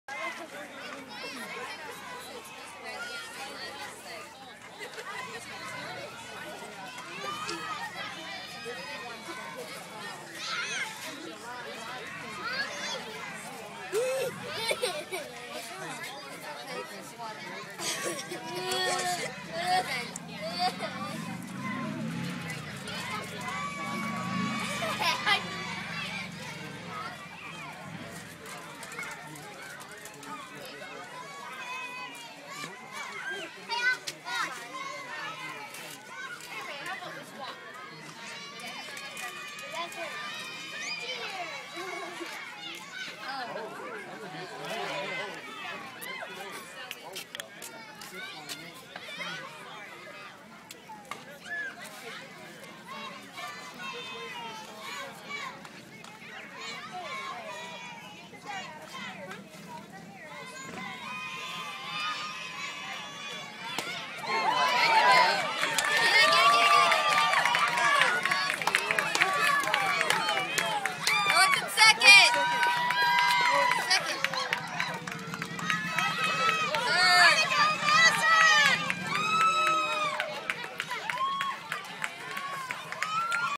Mary Madison's first triple
This is Madison's first year in softball and her first at bat she got a single.  But on her very next at bat she got a triple.  I was a proud daddy when I saw that.